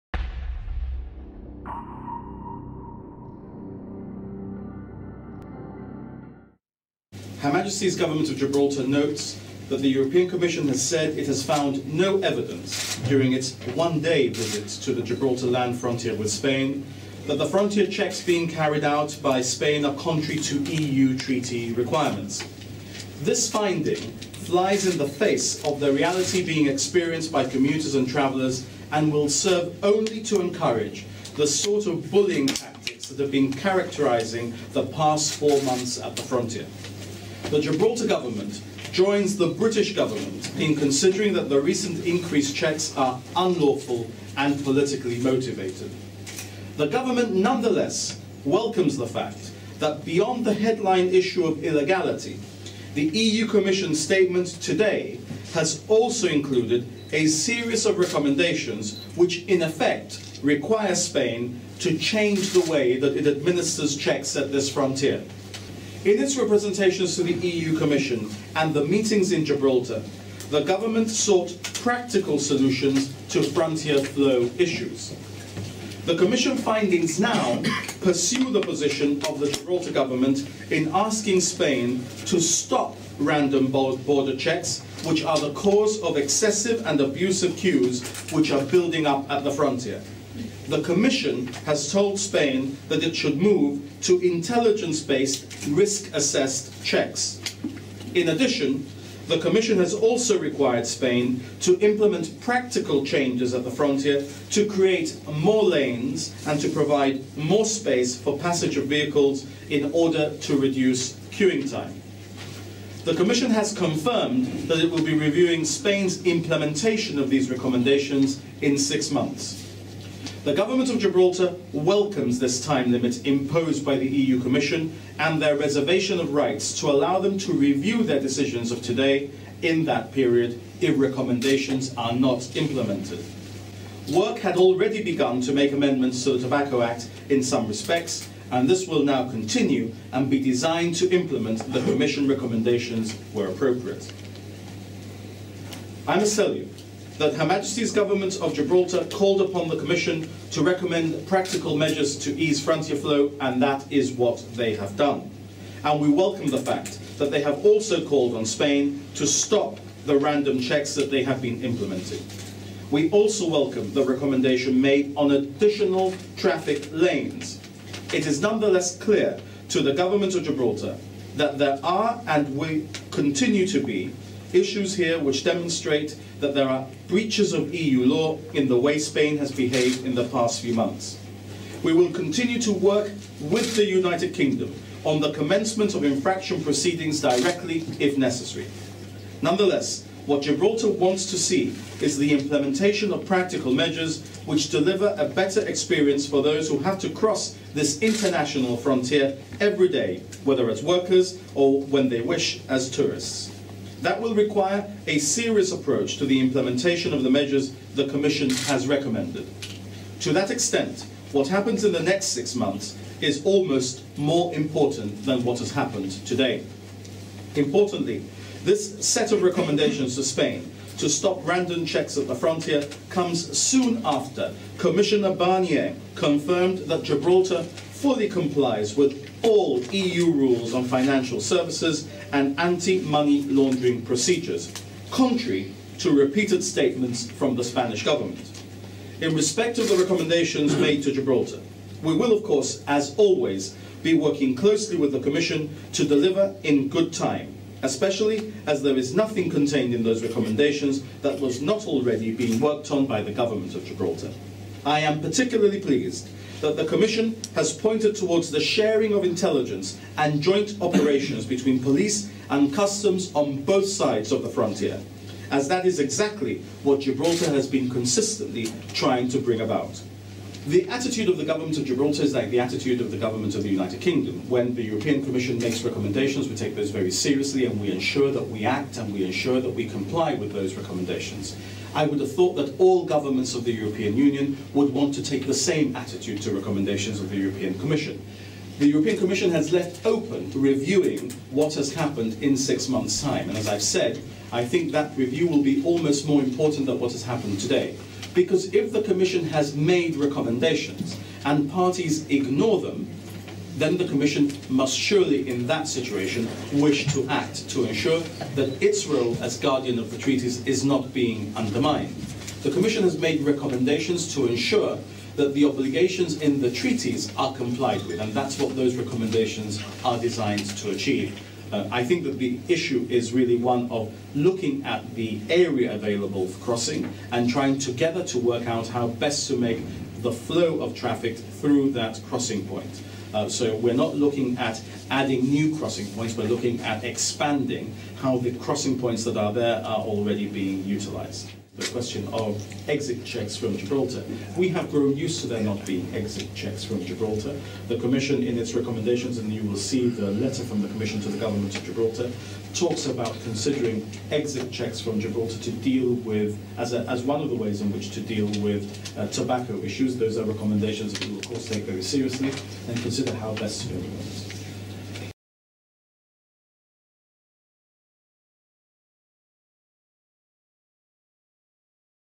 YGTV Gibraltar News Vid: Chief Minister Insists Government Welcomes EU Commission Recommendations
This afternoon Chief Minister Fabian Picardo addressed members of both the Gibraltarian and Spanish press on the Government's response to the EU Commission's statement regarding frontier checks. Mr Picardo insisted that the Government welcomed the Commission's recommendations and insistted the Government looked forward to seeing them inplemented in the next six months.